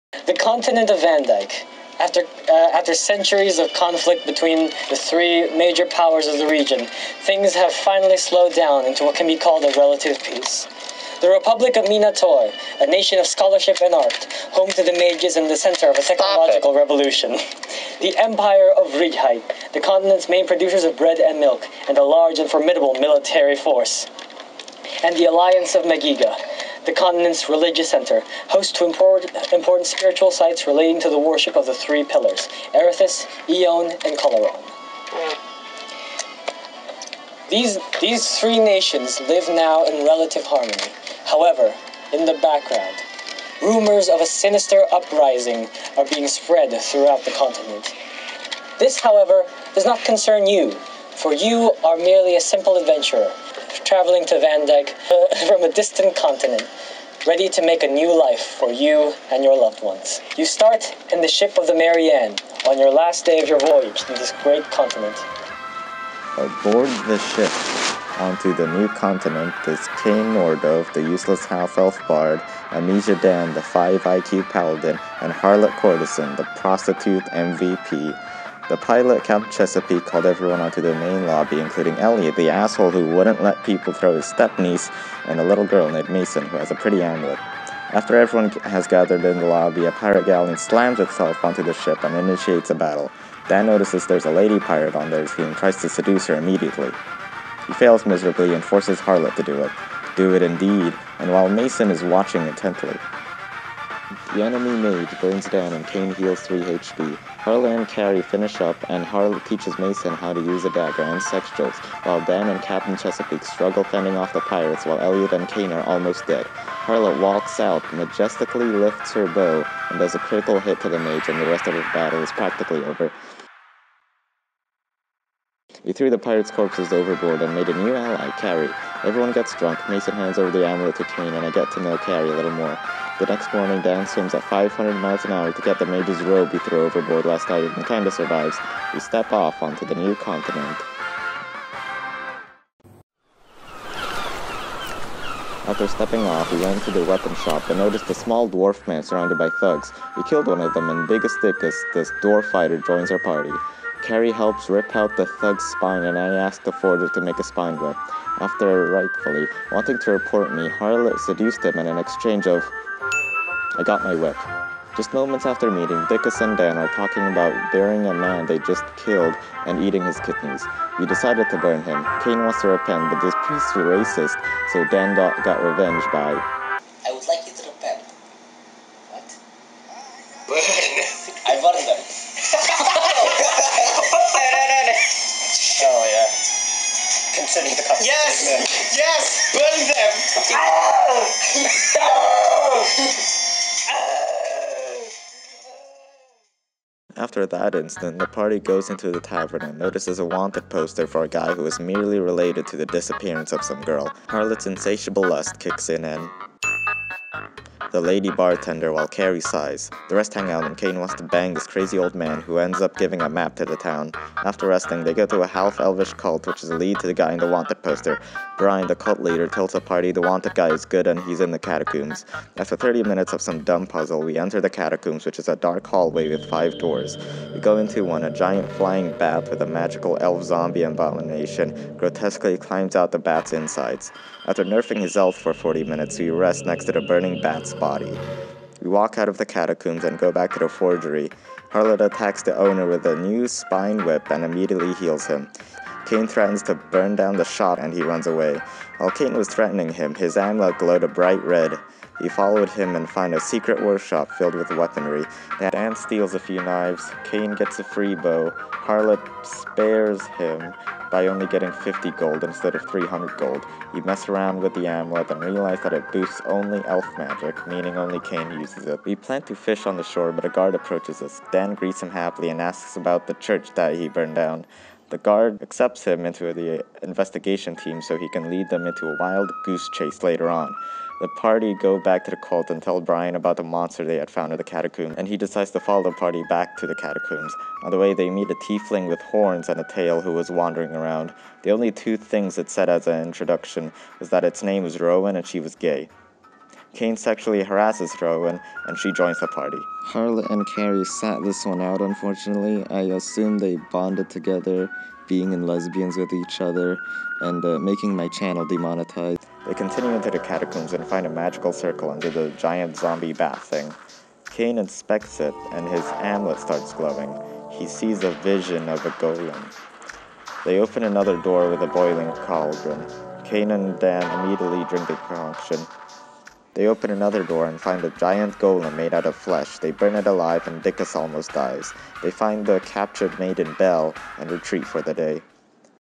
Dnd RECAP: The Wielders of the Everlasting
I uploaded it here again because i feel like i have a right to do so whatre you gonna do a bout it?

Youll find most of the audio there be put into here, but after defeating the zombie monster, we had it on a discord call and trust me you
do not want to hear that. i will "animate" all of it though, what wasnt recorded and whatnot

So about part 2 is completely on a discord call, the audio is absolutely appalling so i will spare you the pain and just animate that part as well.

i also forgot a hilarious part where cains amulet was glowing and theodd1sout shows up with "the dark crystal" and i put his face on it or something AGHAGHAGHAGHGHAH